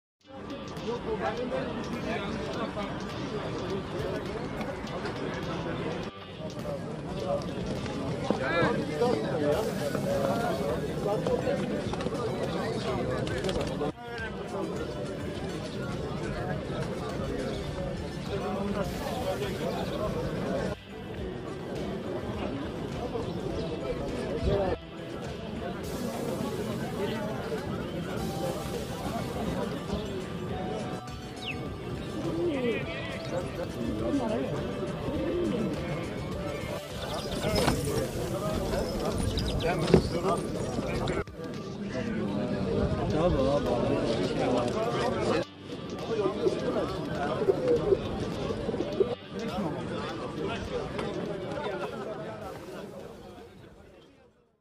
EDİRNEKAPI BIRD MARKET.
istanbul Fatih Topkapı Suriçi Edirnekapı Kuş Pazarı ziyaretimizde yaşadığımız olaylar enstanteneleri derleyip küçük short videosu olarak sunduk. Pazarda saka muhabbet tavuk Horoz ve Kümes hayvanları ile ilgili tükettikleri yemler buğday mısır kenevir istiriyde kabuğu suluk yemlik yavruluk gibi ürünleri satanlarda vardı. Guvercin çeşitleri boldu. Bango Tip Kelebek Mardin Taklacı nuhidi miro arap sütbeyaz kılçıllı hünkarı adana gibi. Güzel ve eğlenceli bir gündü martıların güvercinleri kovalamasına şahit olduk. Köftecisinden pilavcısına kutucusundan satıcısına bir hayli kalabalıktı.